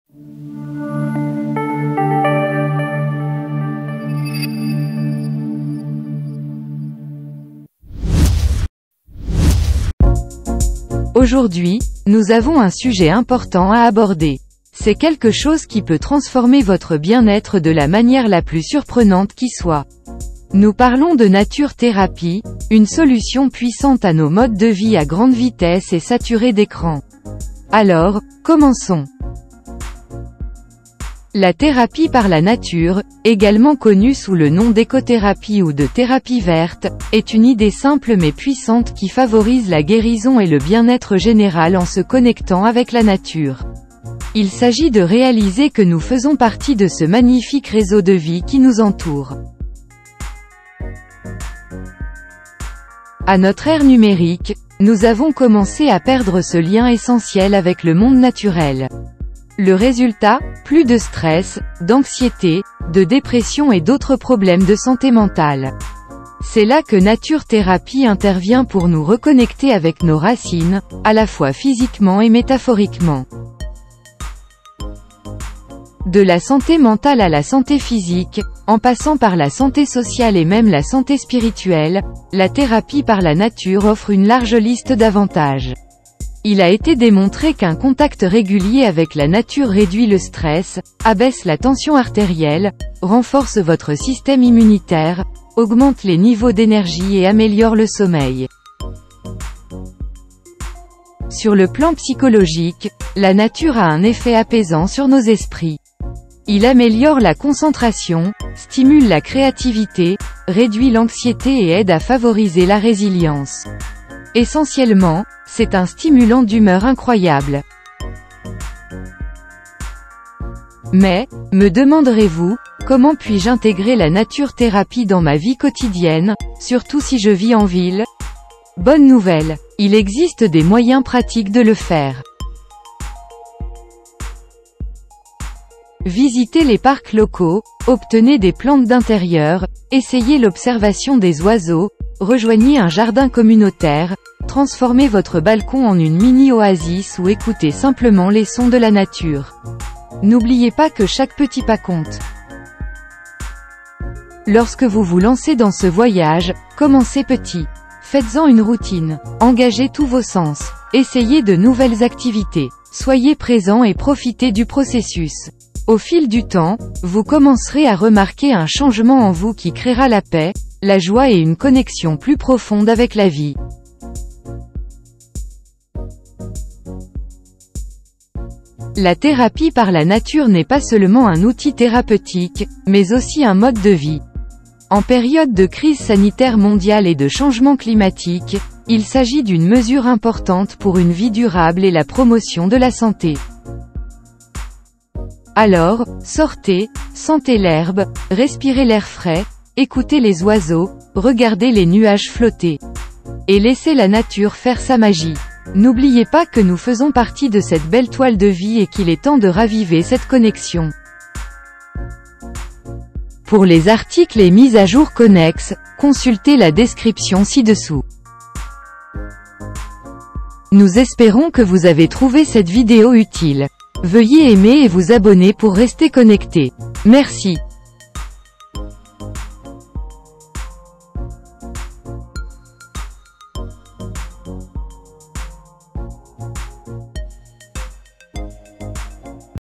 Thérapie naturelle pour le bien-être général
Découvrez le pouvoir transformateur de Nature Therapy. Découvrez comment s'engager avec la nature peut améliorer votre bien-être mental et physique, réduire le stress et améliorer la créativité, même dans les environnements urbains. Découvrez des moyens pratiques d'intégrer la thérapie par la nature dans votre routine quotidienne et faites l'expérience d'un lien plus profond avec la vie. Adoptez la nature et améliorez votre santé de la manière la plus naturelle !